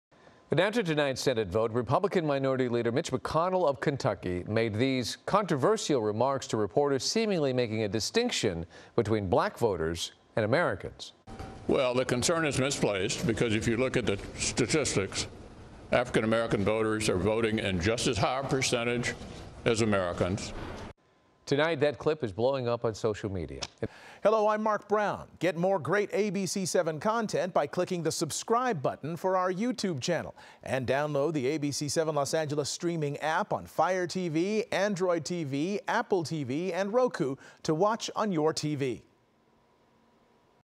Mitch McConnell: Black people vote at similar rates to 'Americans' | ABC7
Senate Republican leader Mitch McConnell is drawing criticism for comments he made shortly before the GOP blocked a federal elections bill, when he said that "African American" voters cast ballots at similar rates to "Americans." Full story